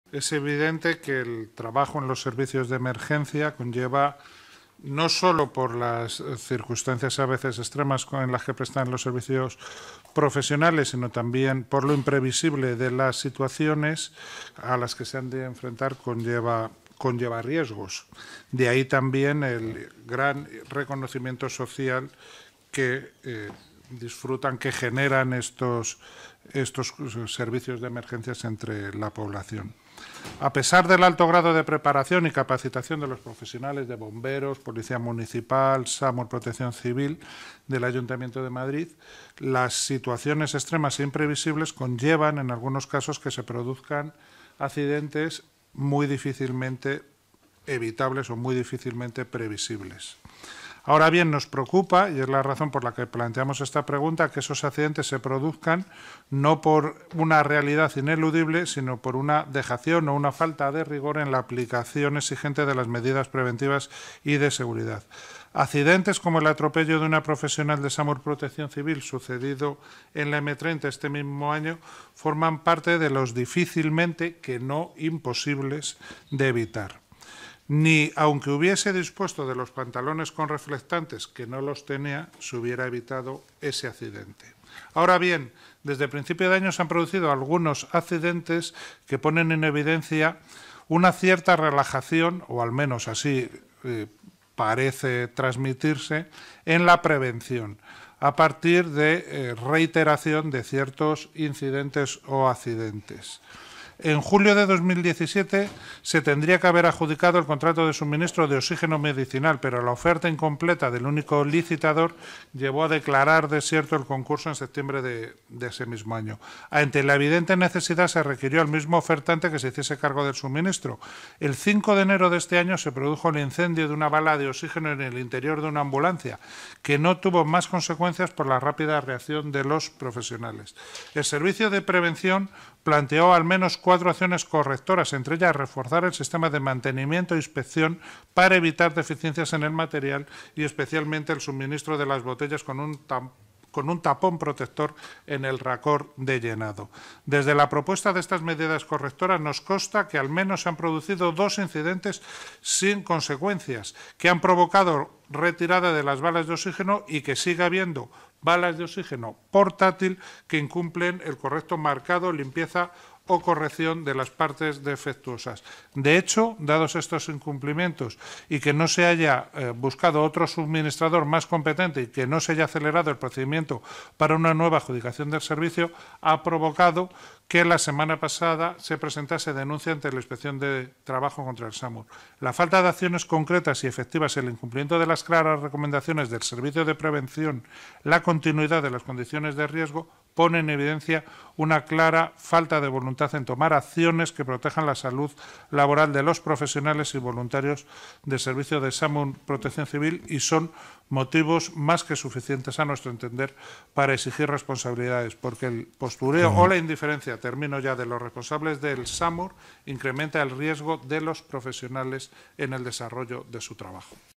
El PSOE advierte “cierta relajación” en la prevención de accidentes en el SAMUR y Protección Civil
Comisión de Seguridad, Salud y Emergencias

• Silva ha preguntado por las medidas adoptadas por el gobierno ante el repunte de la siniestralidad laboral en SAMUR y Protección Civil

• La falta de acciones concretas y efectivas y el incumplimiento de las claras recomendaciones del servicio de prevención “son motivos suficientes para exigir responsabilidades porque el postureo e indiferencia de los responsables de SAMUR incrementa el riesgo de los profesionales en desarrollo de su trabajo”, ha remarcado.
Madrid, 17 de abril de 2018.-

El portavoz socialista en la Comisión de Salud, Seguridad y Emergencias, Ramón Silva, ha advertido “cierta relajación” en la aplicación de las medidas de prevención de accidentes laborales en los cuerpos de SAMUR y Protección Civil. Silva -que reconoce que trabajar en los cuerpos de urgencias y emergencias constituyen una actividad de riesgo por la situación en la que se trabaja y por las situaciones imprevisibles que a veces se producen- ha mostrado su preocupación por los accidentes que se producen por dejación o falta de rigor en la aplicación exigente de medidas preventivas y de seguridad.

El edil, que se ha referido a algunos incidentes recientes quizá evitables, con balas de oxígeno, ha recordado que en julio de 2017 se tendría que haber adjudicado el contrato de suministro de oxígeno medicinal pero la oferta incompleta del único licitador llevó a declarar desierto el concurso en septiembre de 2017 y ante la evidente necesidad se requirió al mismo ofertante que se hiciese cargo del suministro.

Tras un incidente en el interior de una ambulancia el 5 de enero con una bala de oxígeno, el servicio de prevención planteó “al menos” cuatro medidas correctoras. Entre ellas figuraba el refuerzo del sistema de mantenimiento e inspección para evitar deficiencias en el material y especialmente el suministro de  las botellas (de oxígeno) con un tapón protector en el racor de llenado.
Medidas propuestas pero sin efecto práctico porque a Silva le consta después de haber sido planteadas se han producido dos incidentes que han provocado la retirada de las balas de oxígeno, “aunque sigue habiendo balas de oxígeno portátil que incumplen el correcto marcado, limpieza, o corrección de partes defectuosas”

“De hecho, dados estos incumplimientos y que no se haya buscado otro suministrador más competente y que no se haya acelerado el procedimiento para una nueva adjudicación del servicio ha provocado que la semana pasado se presentase denuncia ante inspección del trabajo contra SAMUR” ha añadido.
A su juicio, la falta de acciones concretas y efectivas, el incumplimiento de las claras recomendaciones del servicio de prevención, la continuidad de las condiciones de riesgo pone en evidencia una falta clara de voluntad de tomar acciones que protejan la salud laboral de los profesionales y voluntarios del servicio de SAMUR-PC “son motivos suficientes para exigir  responsabilidades, porque el postureo e indiferencia de los responsables de SAMUR incrementa el riesgo de los profesionales en desarrollo de su trabajo”, ha remarcado. Finalmente, Silva ha indicado que su grupo será intransigente con la desidia de los responsables que ponga en riesgo a los profesionales.